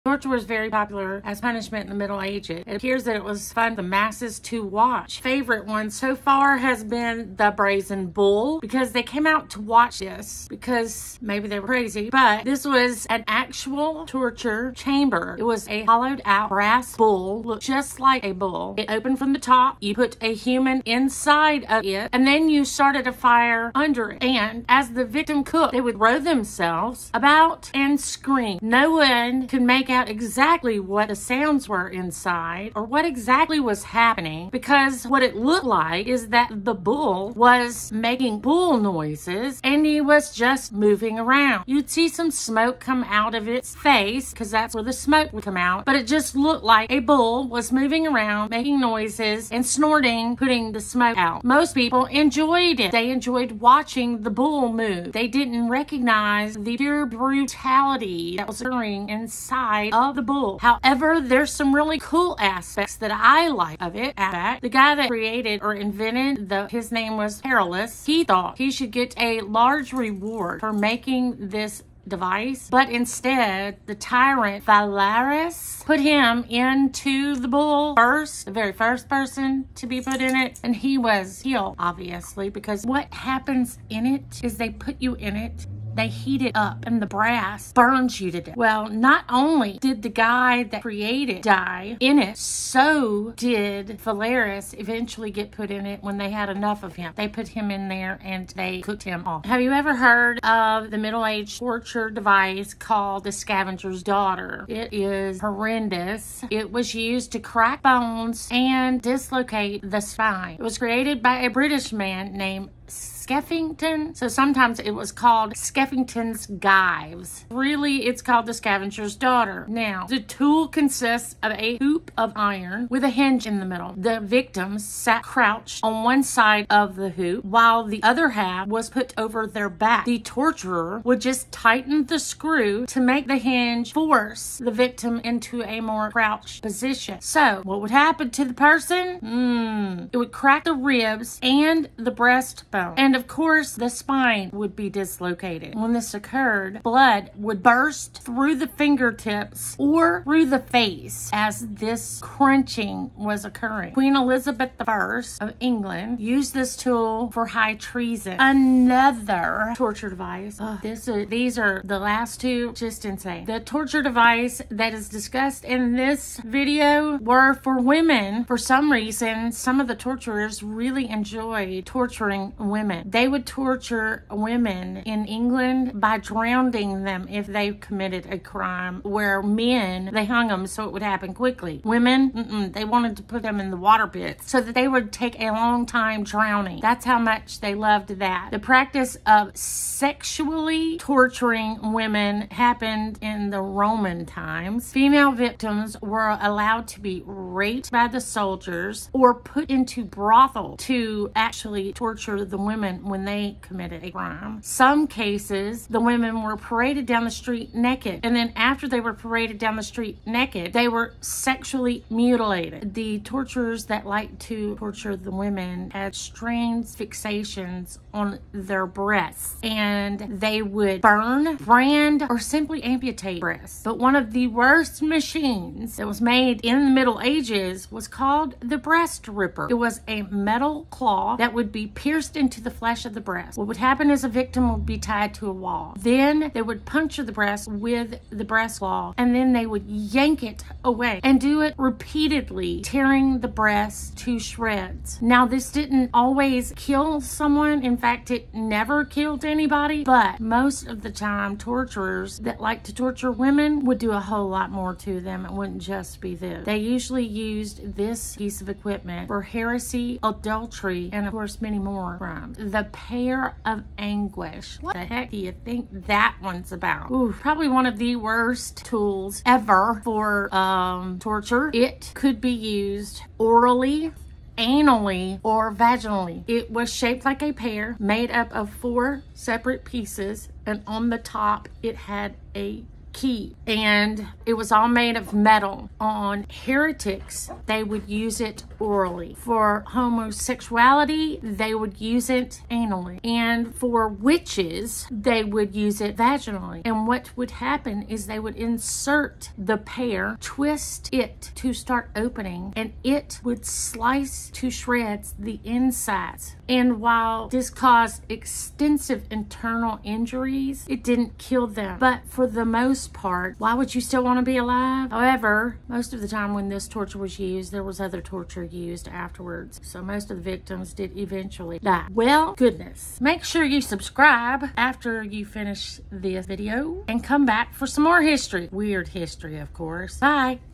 Shocking Facts About Brutal Middle Ages Torture
Step into the dark world of medieval torture methods where the brazen bull reigned supreme. Imagine the terror of being cooked alive inside a metal bull while the crowd watched on